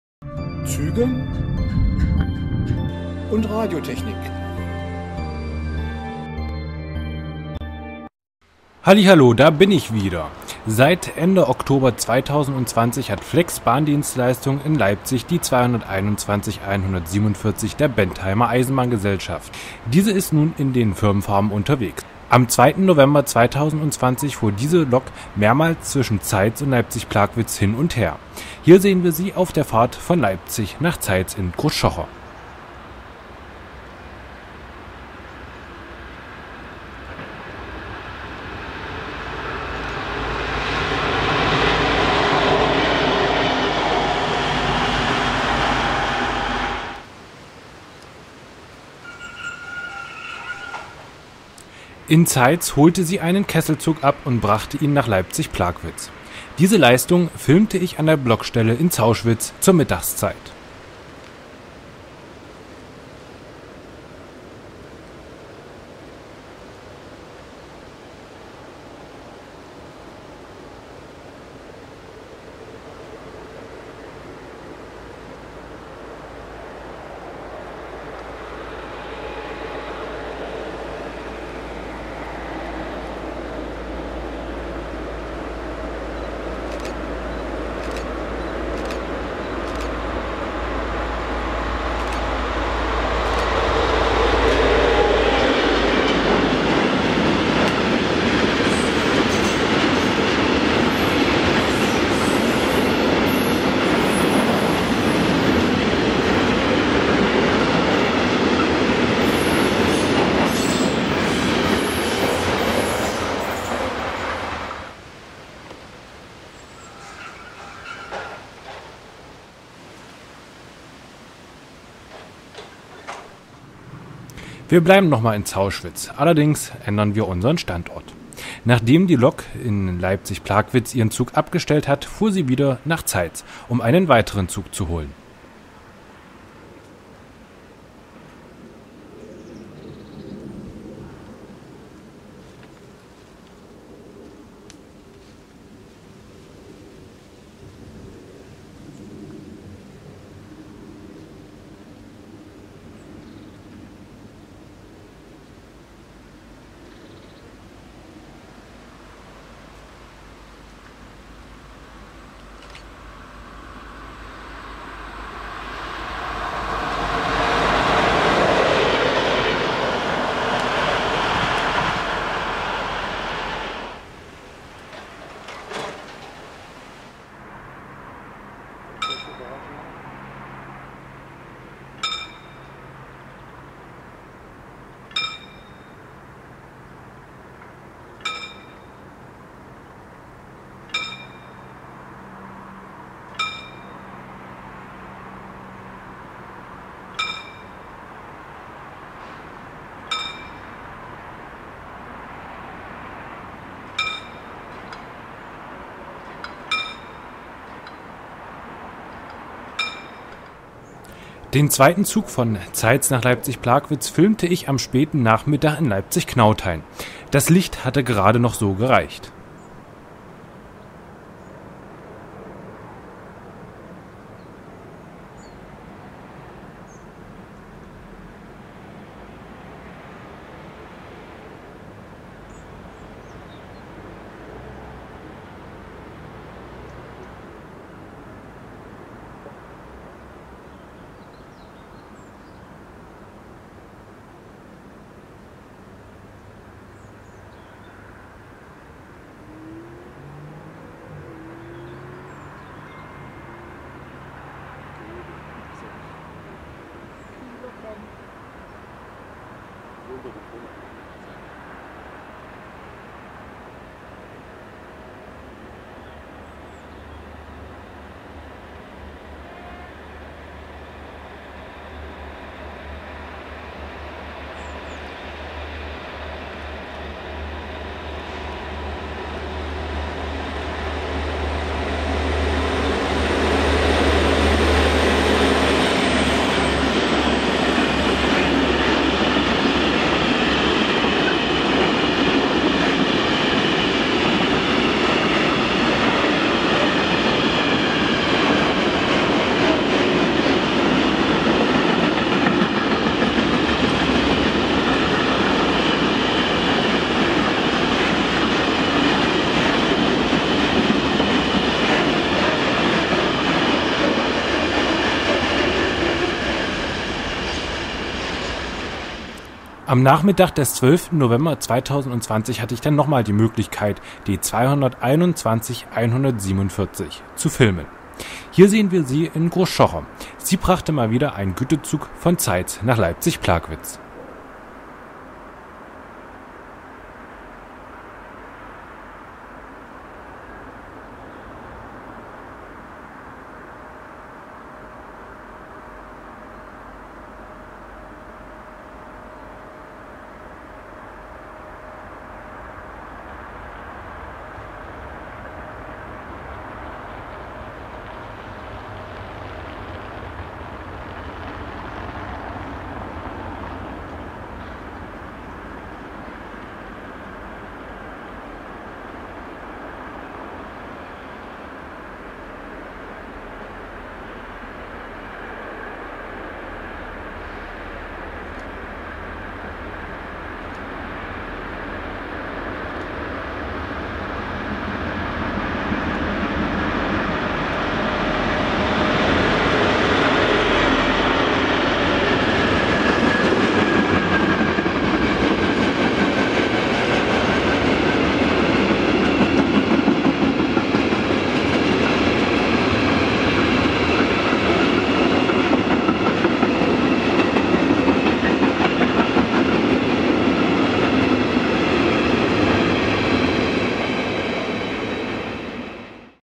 221 147 (Flex) im Einsatz zwischen Zeitz und Leipzig
Leipzig ist nun eine "Attraktion" reicher. Flex Bahndienstleistungen hat seit Ende Oktober 2020 eine neue Lok im Fuhrpark, welche nun regelmäßig vor Güterzügen anzutreffen ist.